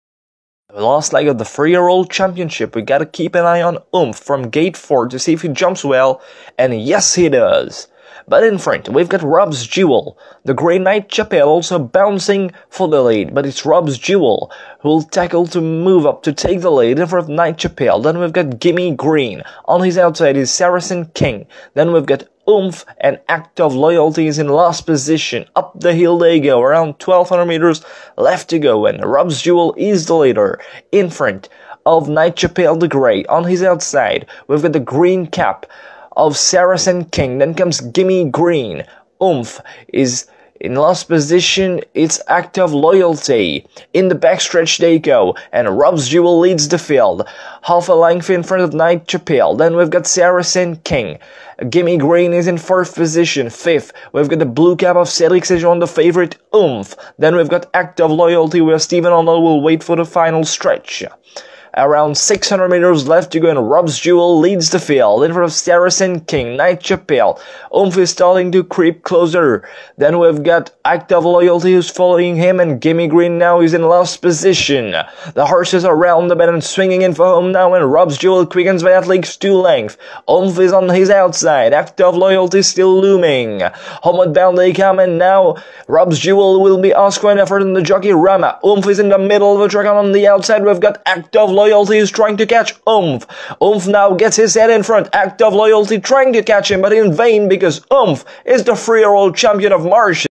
Oomph - The Renault Koleos Trophy (3yo Championship)
Oomph wins the 3 year old championship for Jean Michel Henry stable and for French jockey Cedric Segeon.
Thumbnail Photo: Nitish Equine Photography